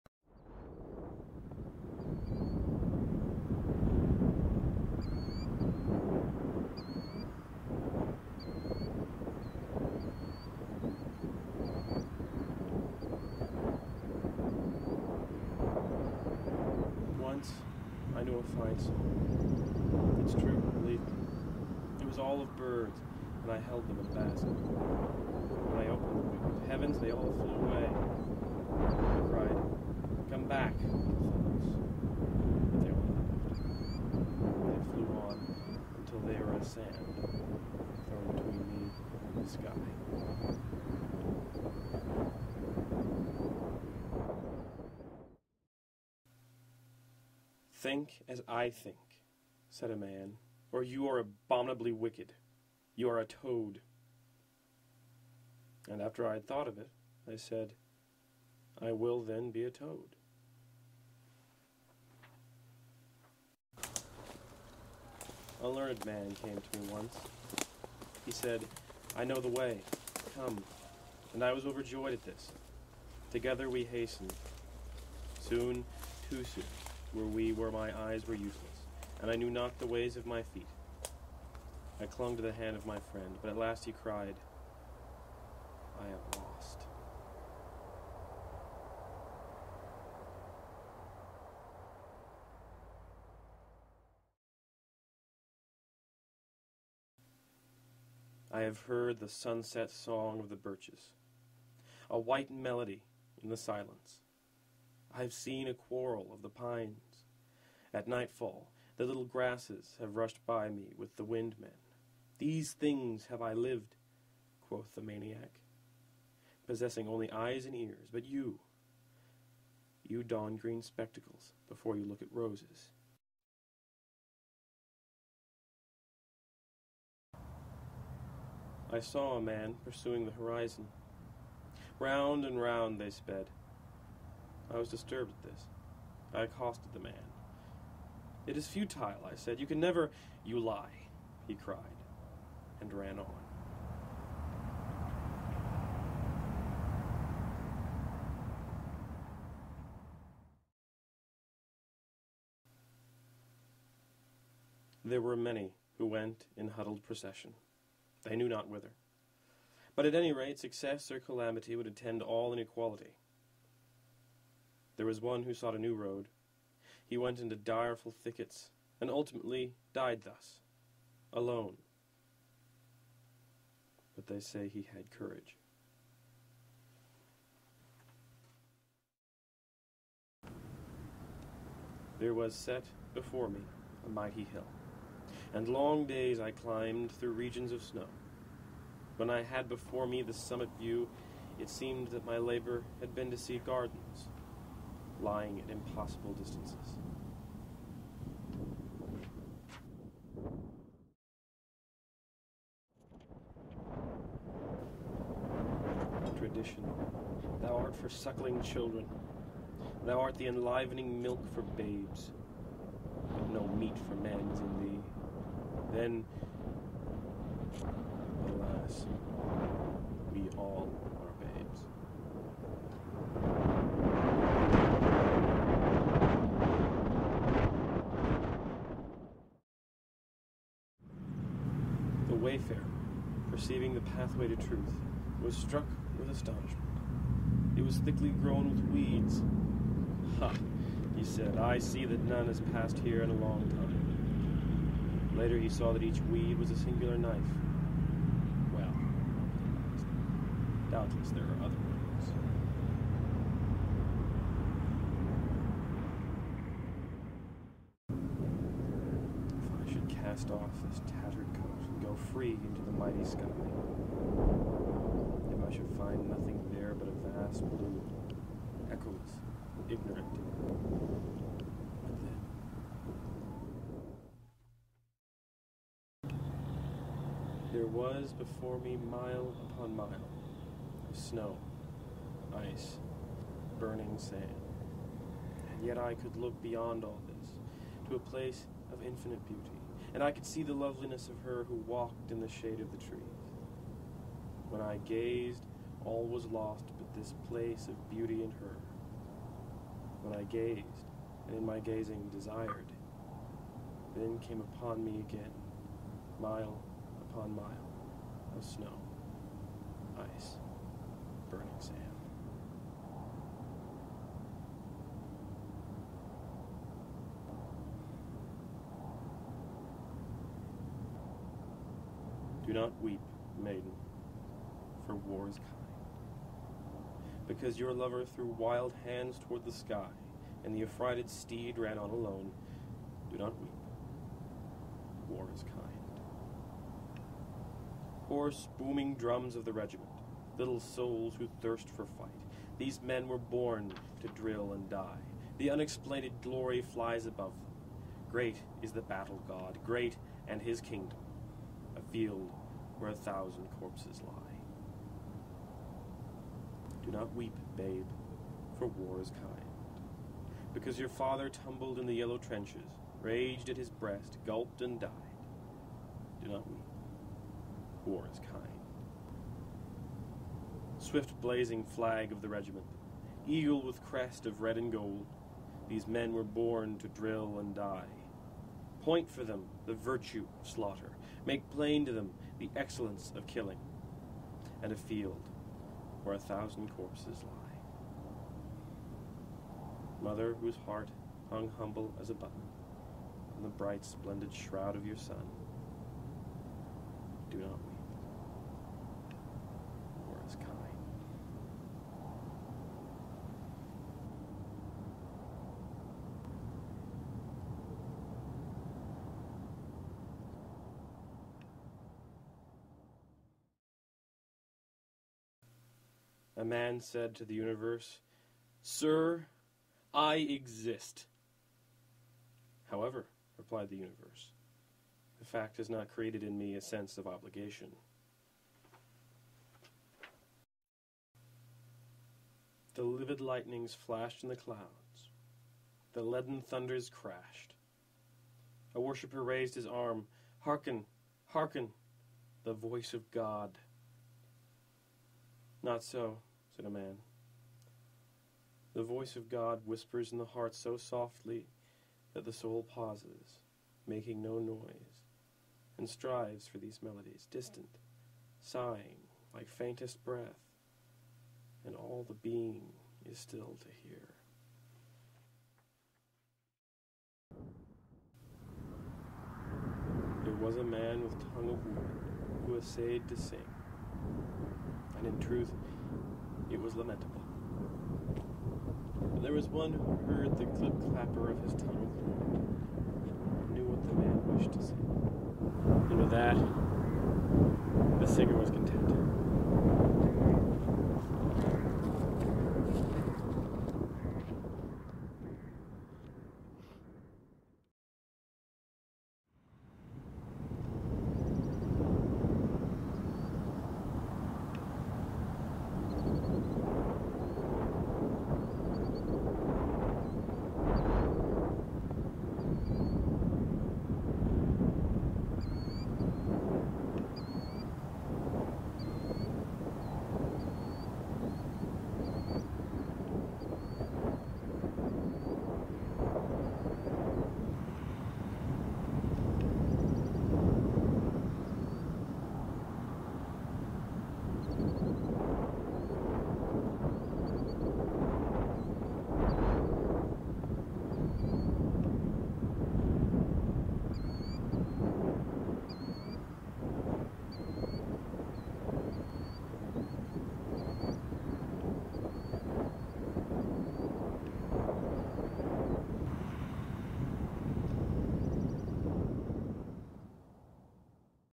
Once, I knew a Fine Song: The Poetry of Stephen Crane
A short film constructed of Stephen Crane poems.